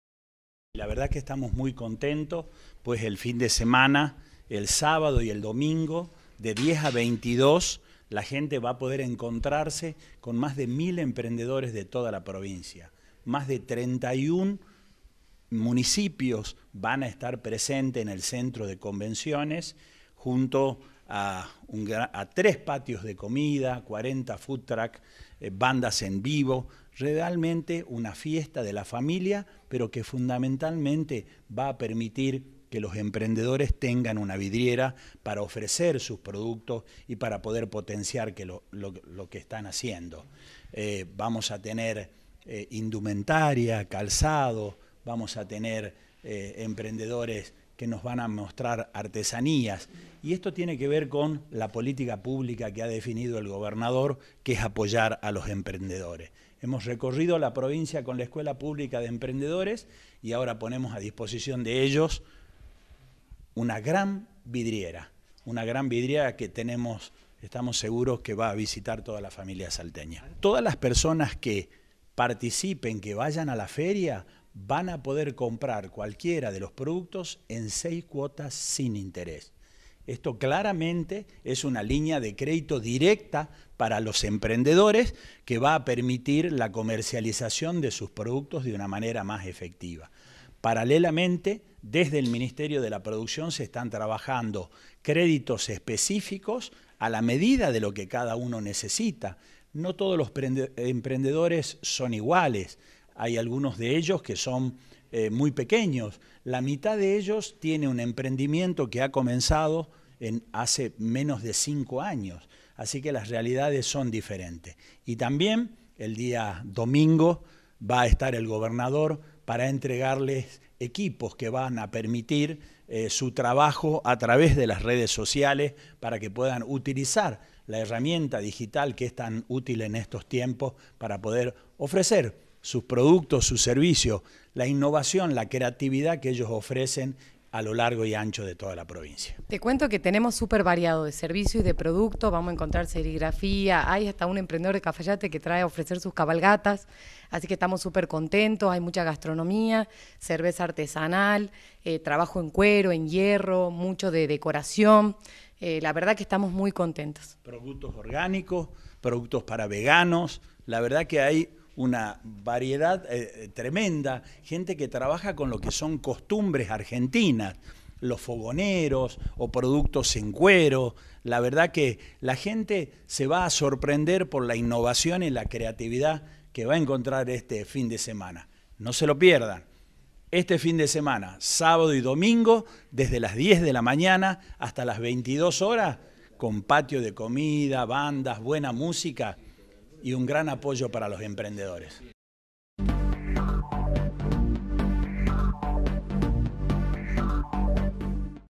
SALTA - Alrededor de mil emprendedores participarán en la Feria Potencia Emprende 2022
La jornada que se realizará en el predio del Centro de Convenciones Salta, se desarrollará este sábado y domingo de 10 a 22 y contará con tres patios de comida, 40 foods trucks y bandas en vivo. La entrada es libre y gratuita.